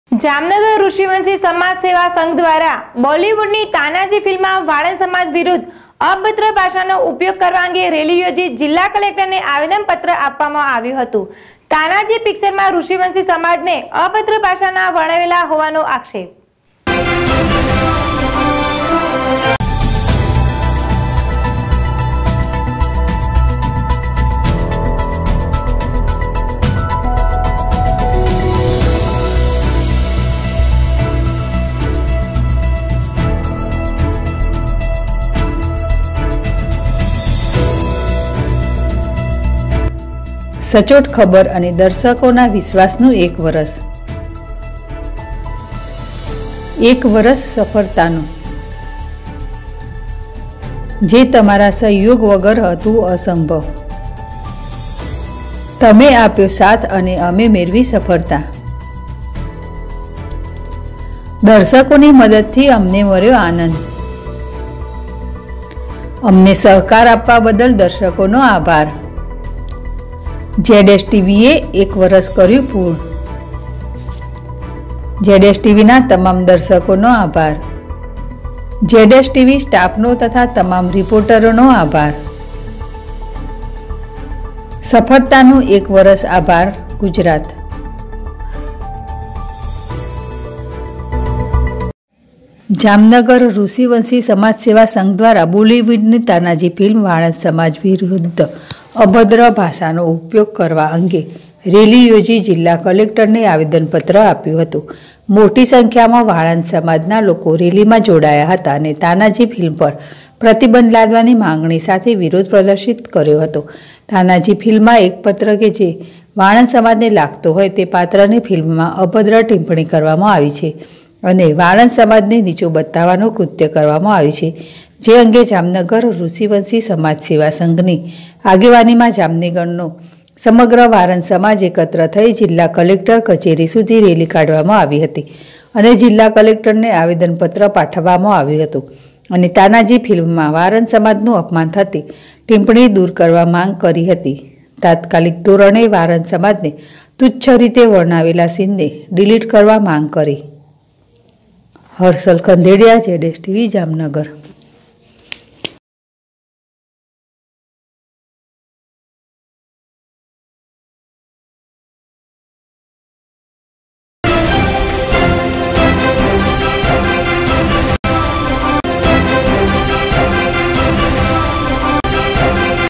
જામનગરમાં ઋષિવંશ સમાજ દ્વારા તાનાજી પિકચરના વિરોધમાં વિશાળ રેલી કાઢવામાં આવી.
જામનગર માં ઋષિવંશ સમાજ દ્વારા તાનાજી પિકચરના વિરોધમાં વિશાળ રેલી કાઢવામાં આવી.